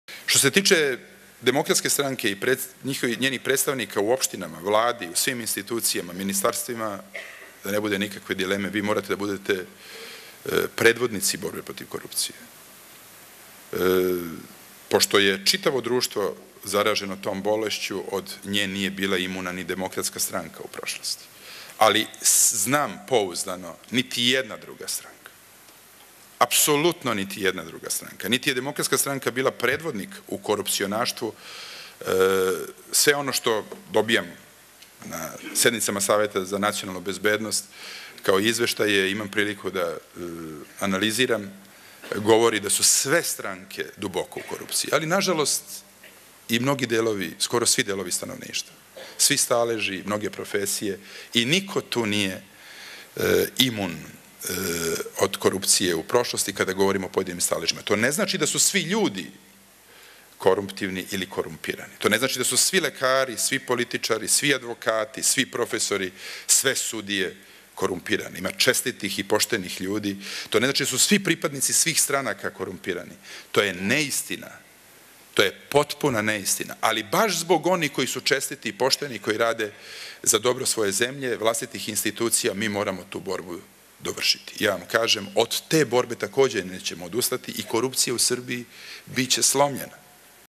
Boris Tadić na sednici Glavnog odbora DS (5/6)
(Beograd, 30. maja 2009) Obraćanje predsednika Demokratske stranke i predsednika Srbije Borisa Tadića na sednici Glavnog odbora DS. (za više informacija posetite